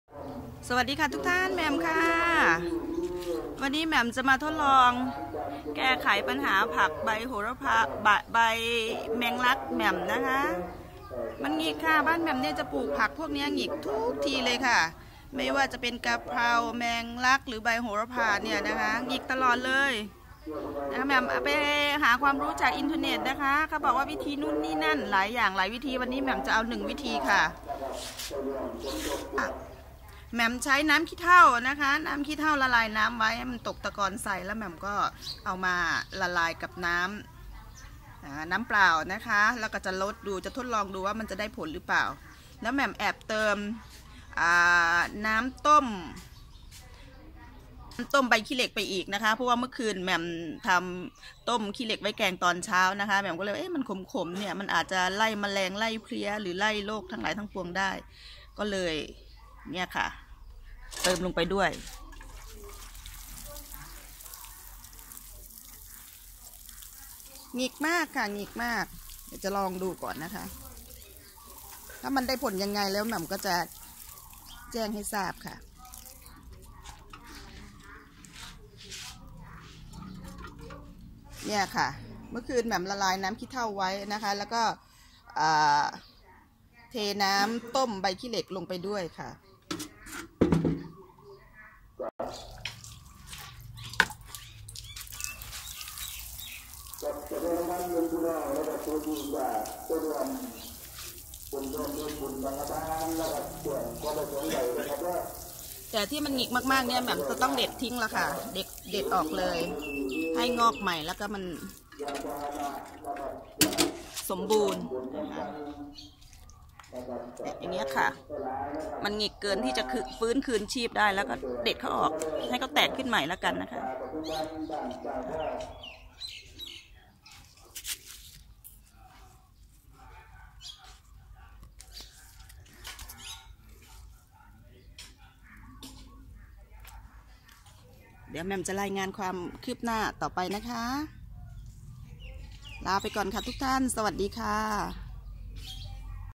ทดลองสูตรรักษาโรคใบหงิก แมงลักกระเพราโหระพาEp1 24/7/2552
แหม่มทดลองแก้ไขรักษาโรคใบหงิกของกระเพราใบแมงลักและโหระพา ด้วยน้ําด่างจากขี้เถ้าและน้ำต้มจากใบขี้เหล็กค่ะ